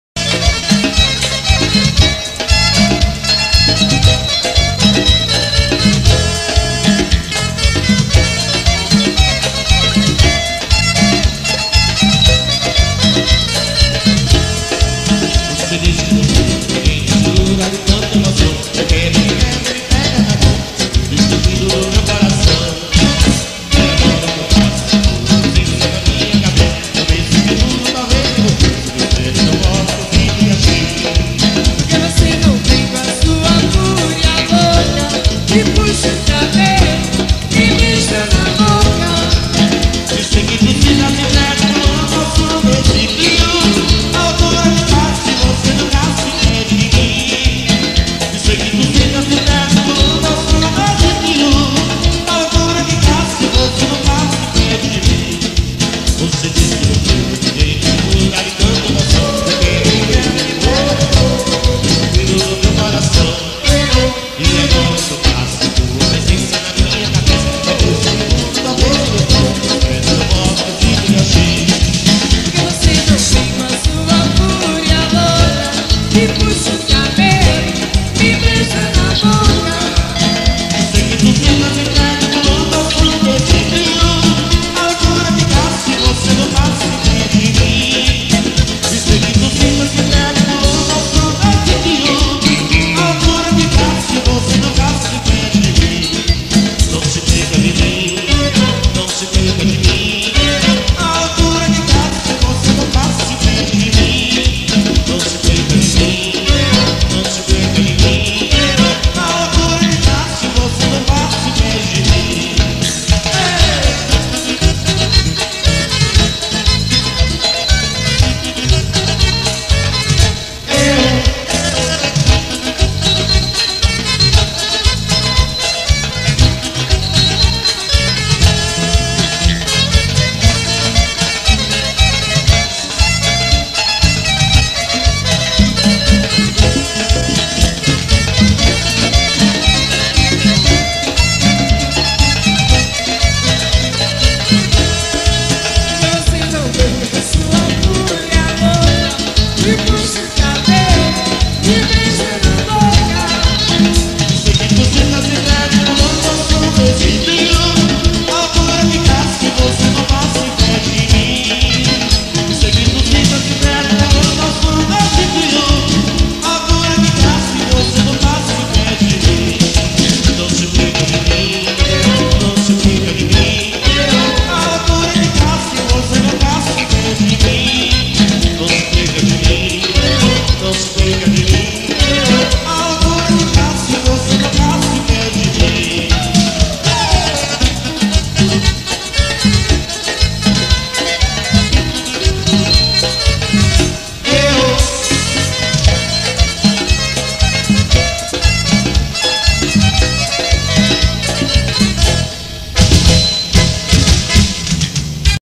Aracaju - Sergipe 1992
Banda Valneijós com Cinho e Paulinho em Aracaju - Sergipe